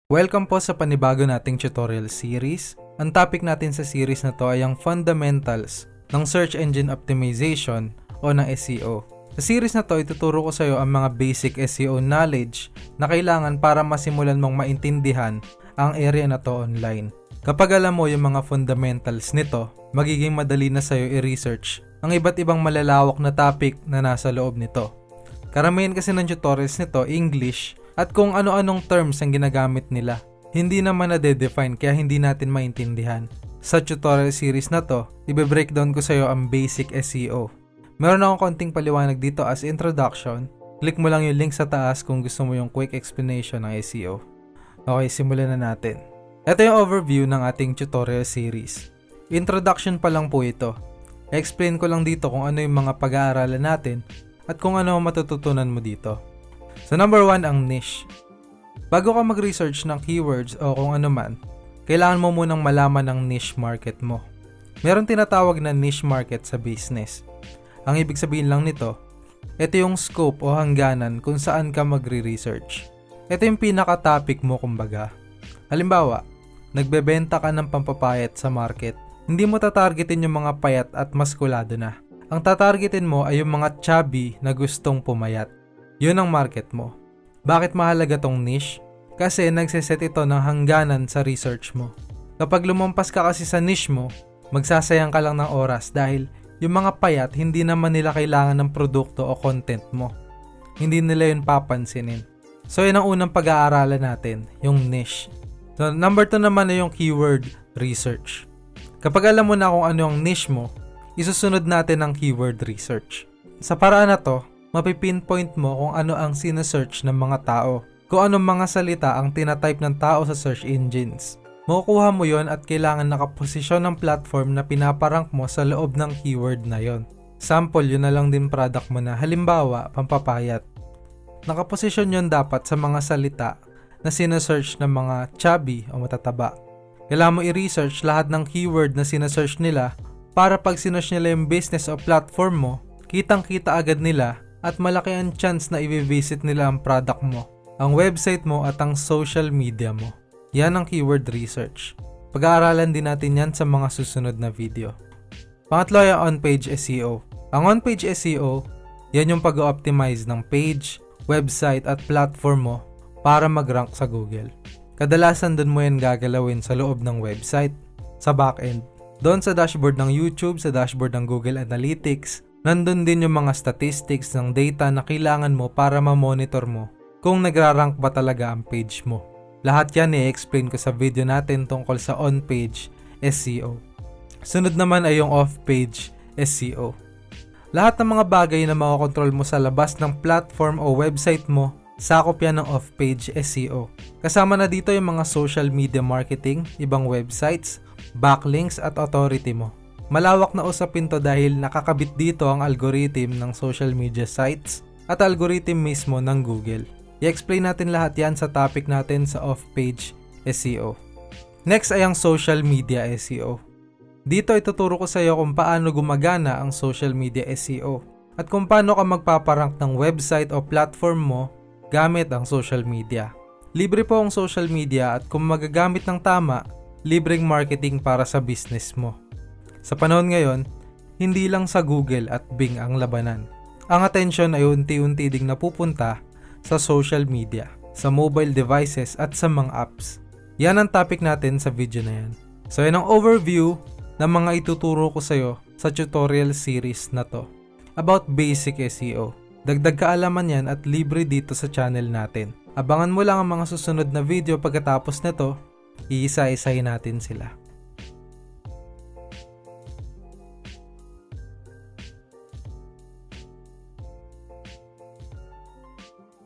Part 1: BASIC SEO Tagalog Tutorial | INTRODUCTION | Illustrados
Sa video tutorial series na 'to, ituturo ko sayo ang mga basics kung paano gumagana ang search engine optimization at ito ang ating introduction. Overview po ito ng mga pag-aaralan natin dito sa series.